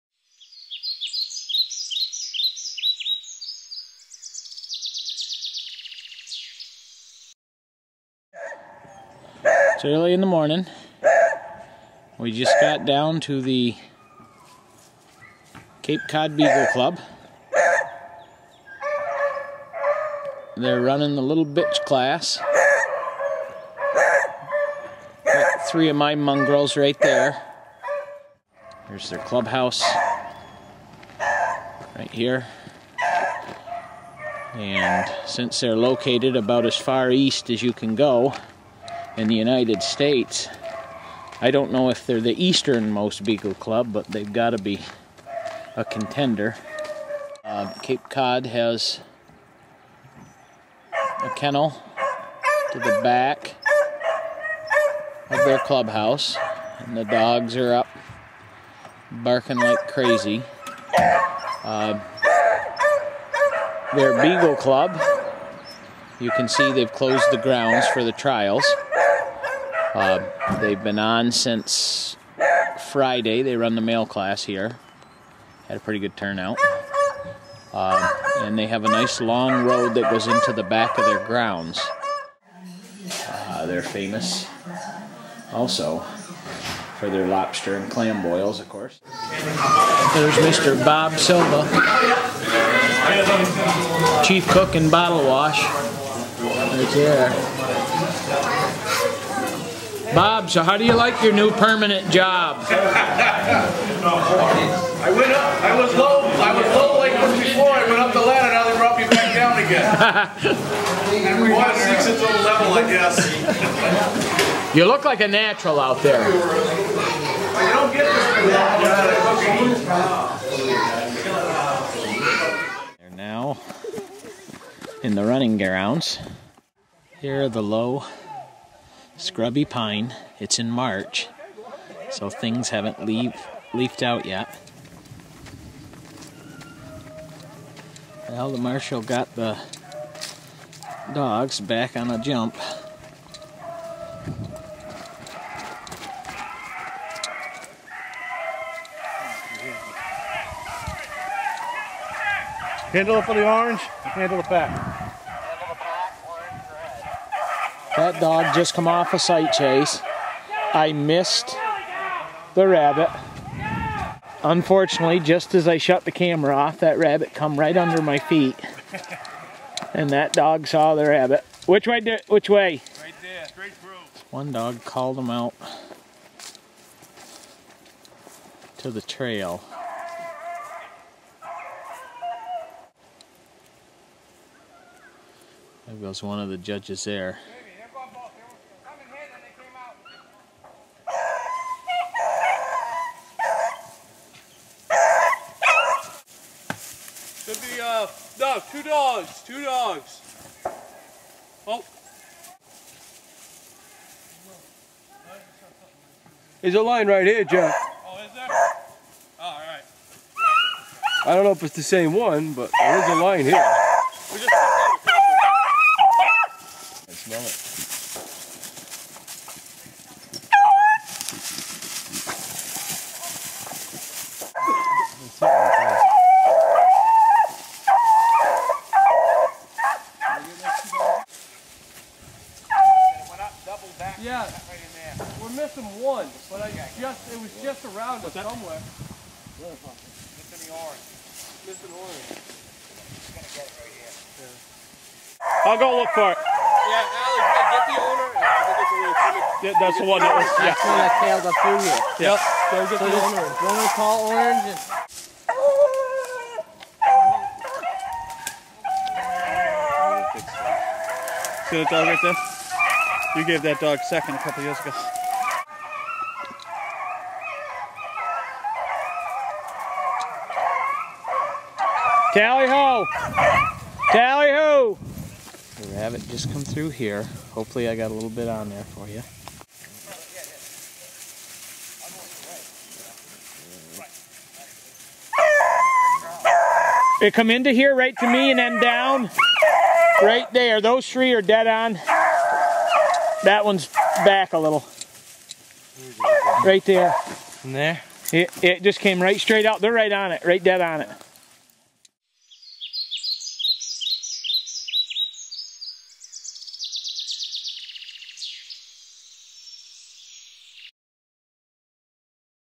Cape Cod Beagle Club SPO Trial 3/20/11
This Small Pack Option Beagle Field Trial shows the 13" female class running on March 20, 2011 narrated by Ralph Harrington, editor of Better Beagling Magazine in Falmouth, Massachusetts.